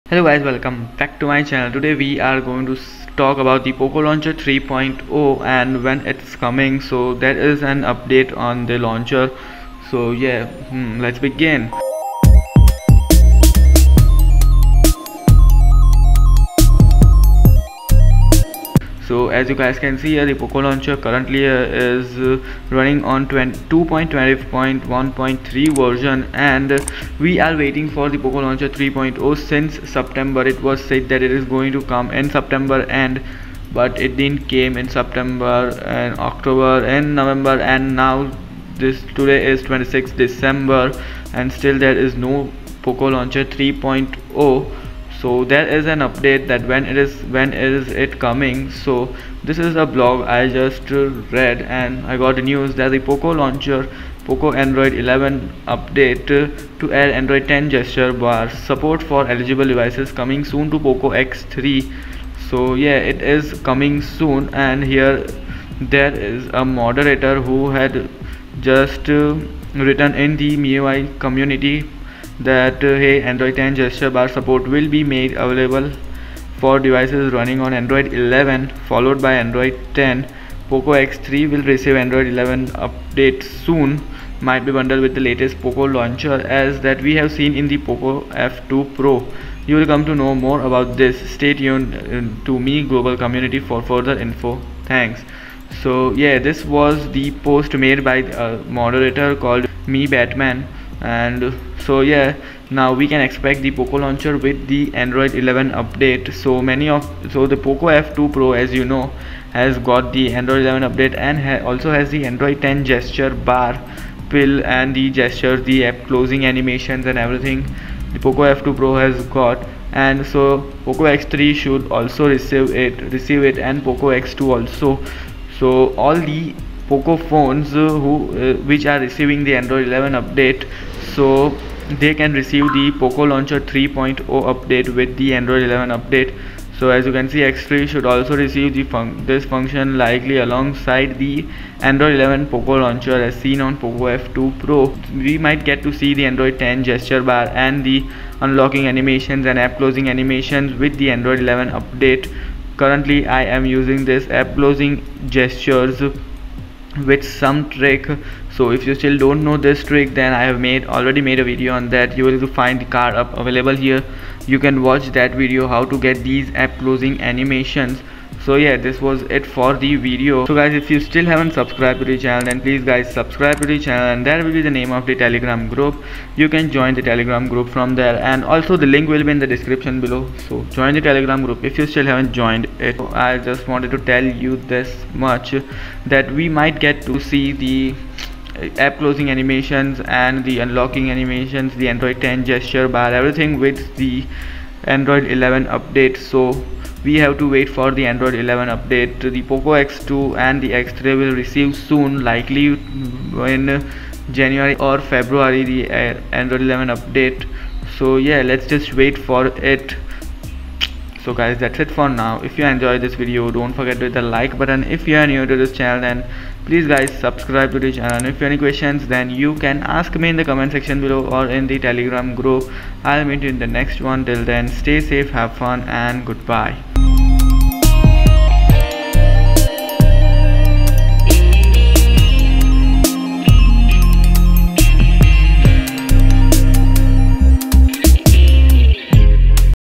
POCO Launcher 3.0 Update Release Date | New Animation, Gesture Bar Coming soon
This video is about the Poco Launcher 3.0 Release Date which will have the Android 10 Gestures Bar, New Animations and many more. Please watch the video till the end to know more about the poco launcher update.